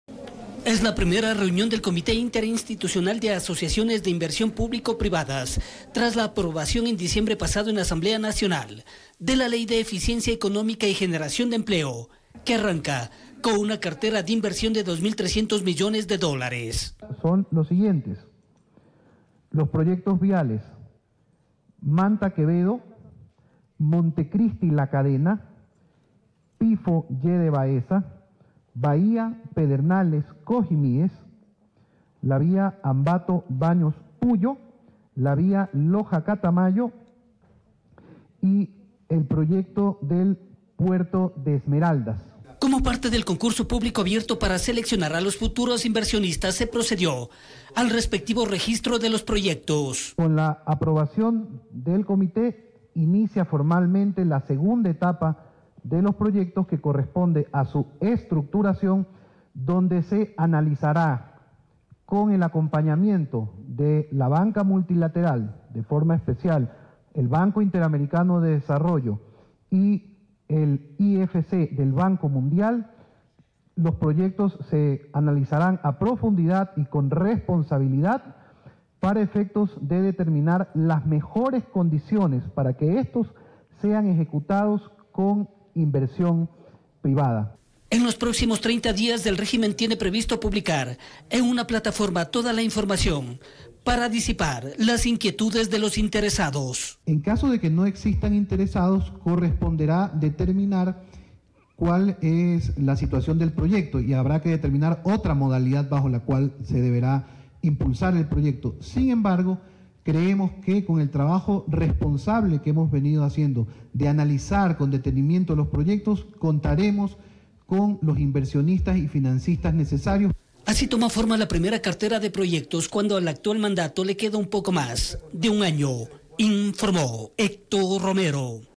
Proyectos viales y marítimos se lograrían gracias a la inversión público-privada
Mecanismo busca evitar los actos de corrupción en la contratación pública.